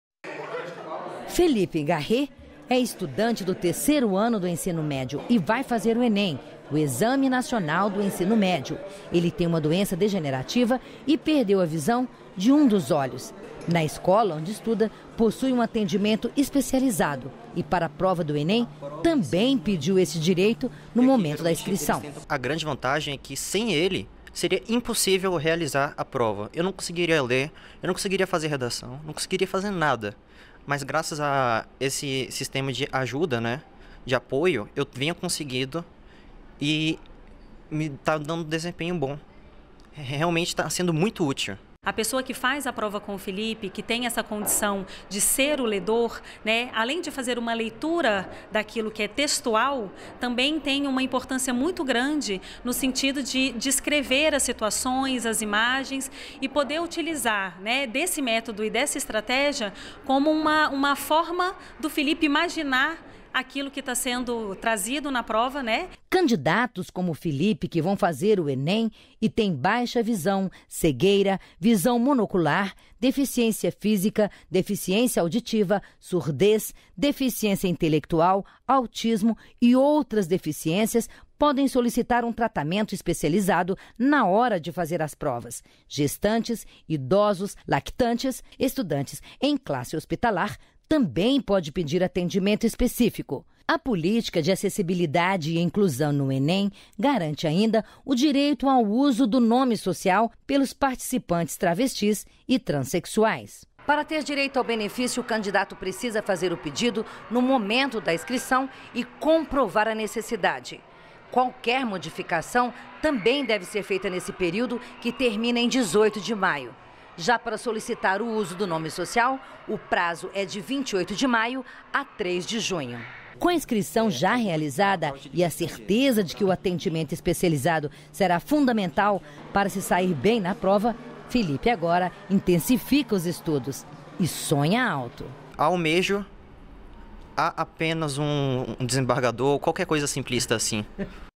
Candidatos que precisam de atendimento especial no Enem devem fazer o pedido no ato da inscrição
REPÓRTER NBR - 16.05.18: inscrições para o Exame Nacional do Ensino Médio, Enem, terminam na próxima sexta-feira, 18 de maio. E os candidatos que precisam de atendimento especial, como as pessoas com deficiência, devem fazer esse pedido no momento da inscrição.